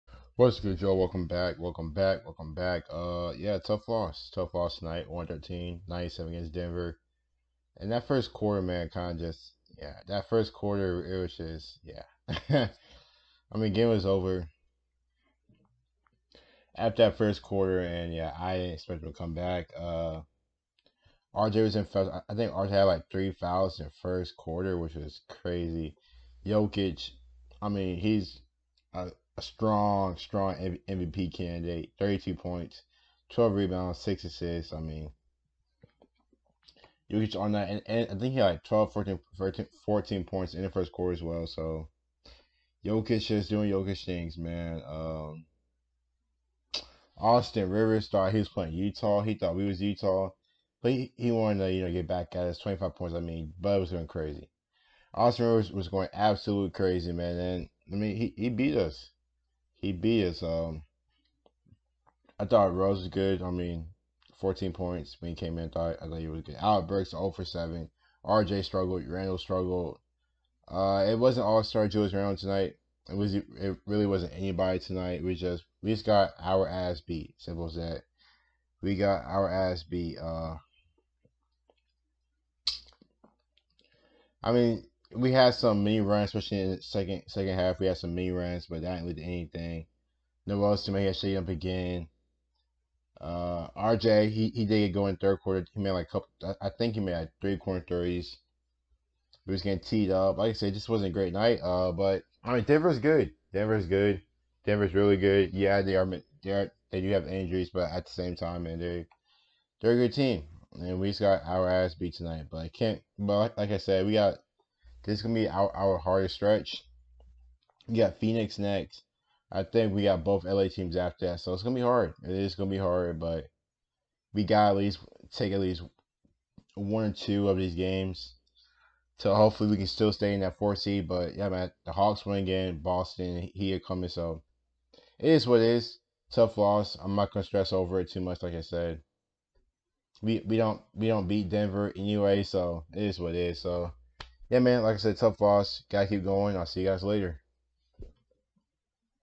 NEXT GAME...KNICKS 97 NUGGETS 113 NBA POST GAME REACTION
Tough game... got to move on!!!